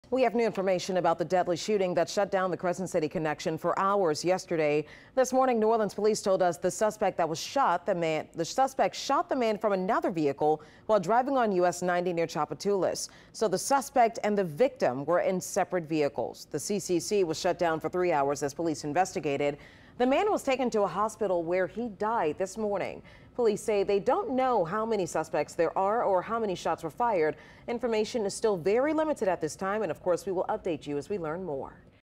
New info on Crescent City Connection shooting
WWL Louisiana has new information about the deadly shooting that shut down the Crescent City Connection for hours on Thursday.